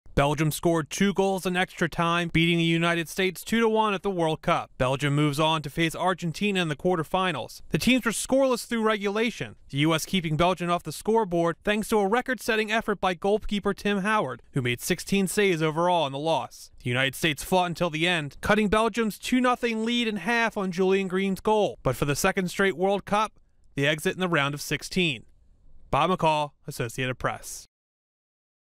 US World Cup Team Loses, Done in Brazil
Belgium scored two goals in extra time, defeating the United States 2-1 at the World Cup. Belgium will face Argentina in the quarterfinals. (July 1)